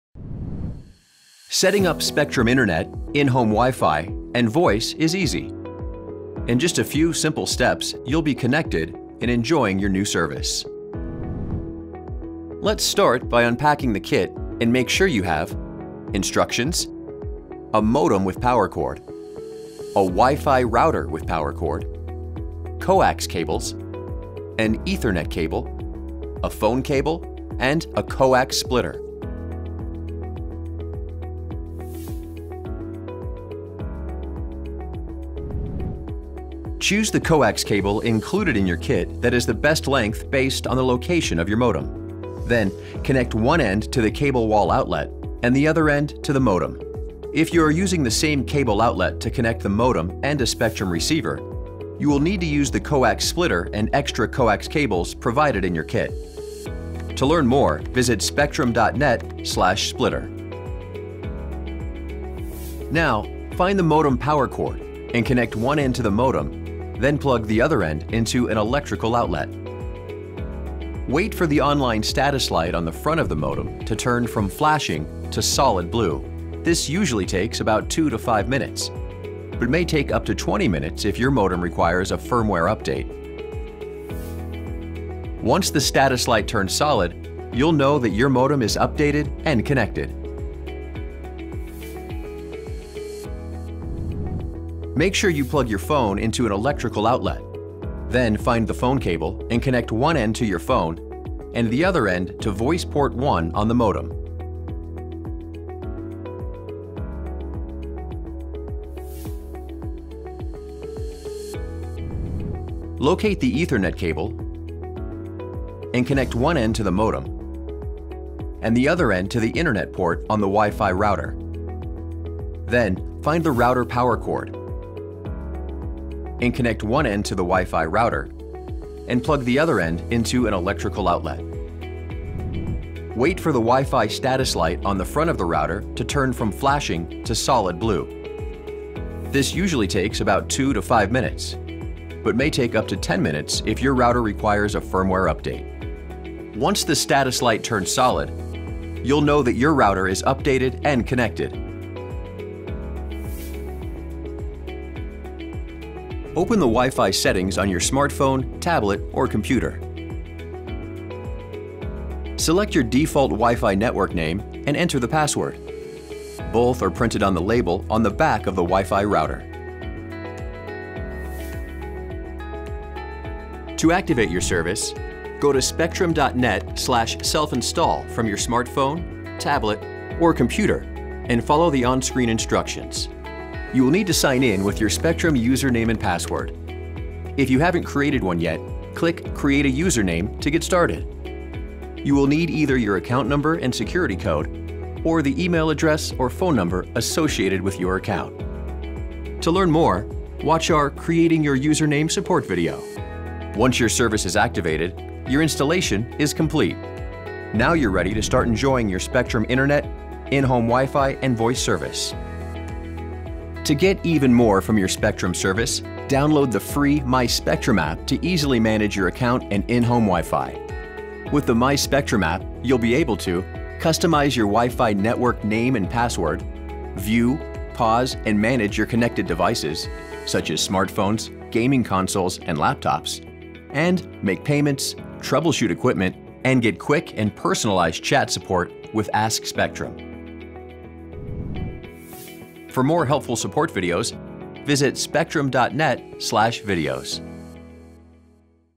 Self-Installation: Internet, WiFi & Voice
Self-installation is quick and easy. In just a few simple steps you’ll be set up and enjoying your new Internet, In-Home WiFi and Voice service. This video will take you through the self-installation kit and install process step-by-step.